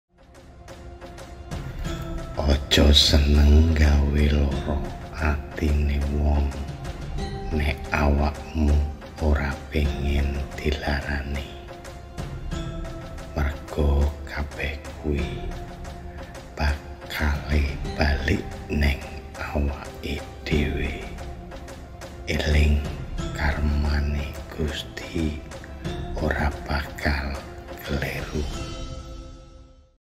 Story WA Jawa Bijak 30 detik - Eling‼️ | Pitutur Jowo Menyentuh Hati Kanggo Nglakoni Urip
Salam Rahayu Sedulur..Ini adalah video story wa jawa bijak 30 detik, tentang pitutur jawa yang luhur dan menyentuh hati.

"OJO SENENG GAWE LARA ATINE WONG,
NEK AWAKMU ORA PINGIN DILARANI.
MERGA KABEH KUI
BAKALE BALIK NENG AWAKMU DEWE
ELING, KARMANE GUSTI ORA BAKAL KLIRU ..!!!

Sumber Gambar : IG : diansuryo1812